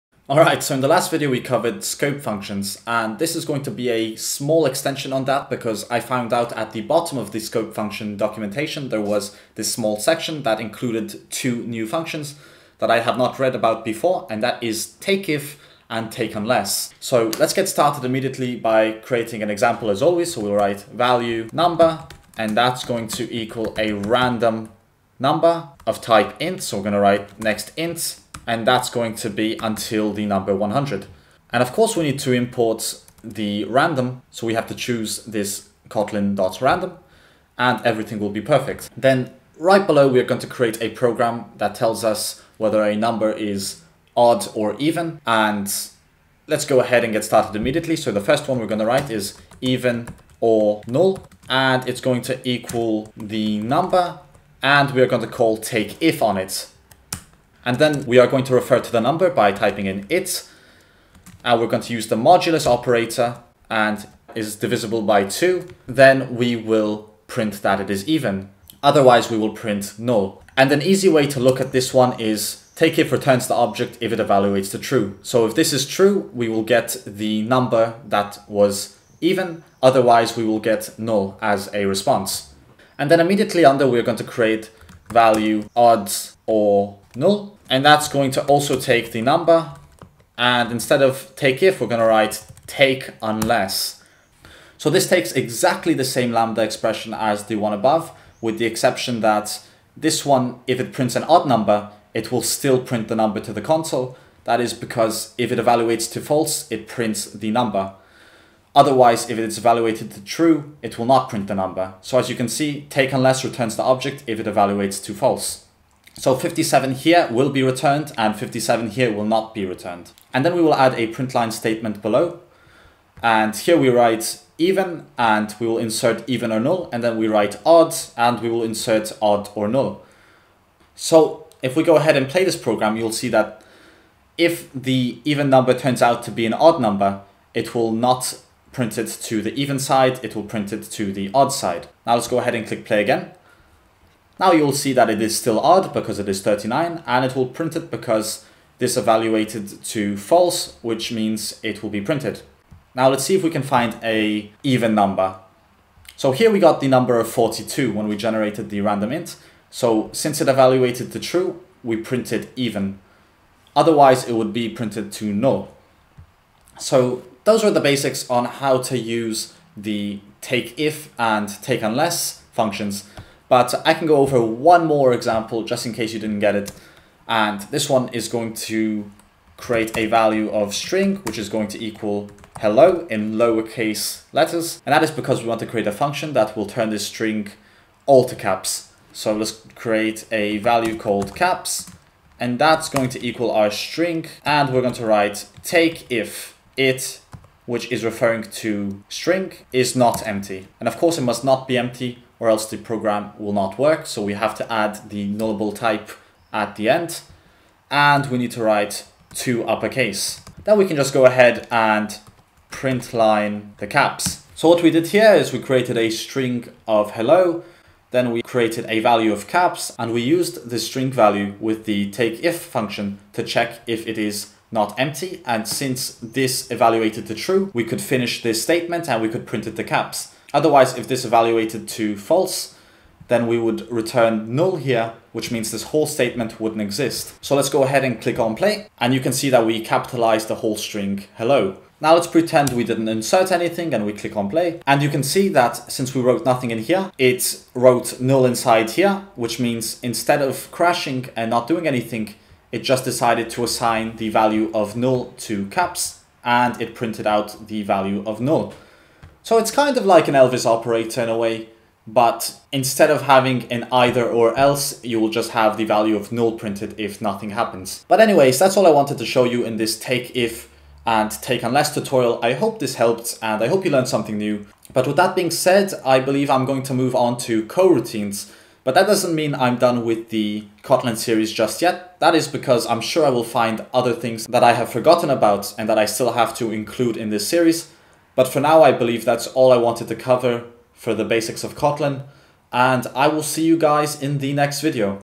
Learn Kotlin for Android: takeIf & takeUnless (Lesson 27)
In this video I will be showing you how to use takeIf and takeUnless in Kotlin.